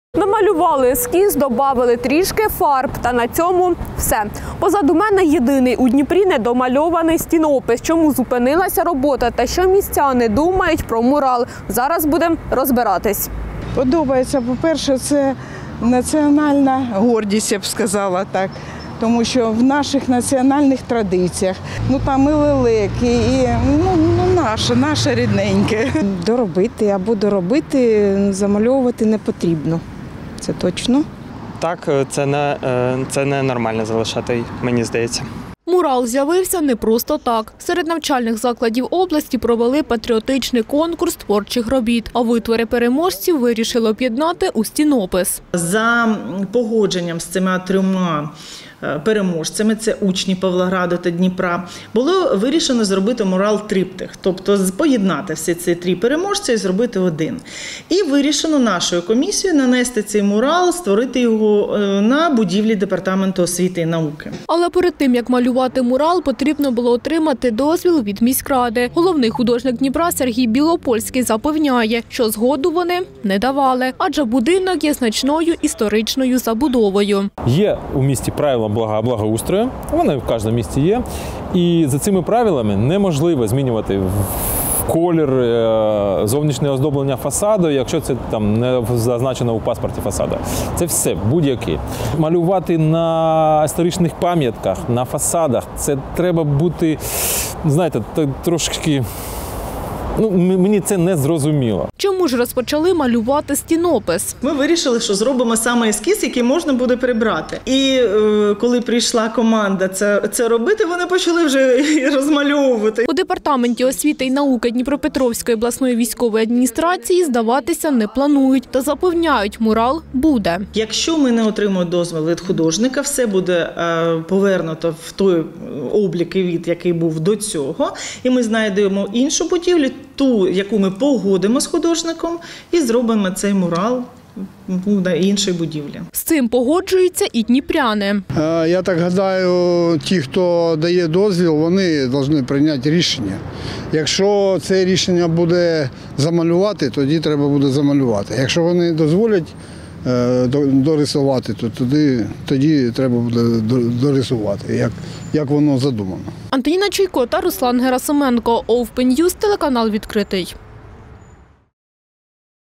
Чому у Дніпрі з’явився недомальований мурал?
Лелеки, квіти та козак. На одній з будівель Дніпра почали малювати стінопис. Але процес призупинили. Бо ж у міськраді запевняють, дозволу на нього - не давали. Чому так вийшло? Та чи завершать мурал?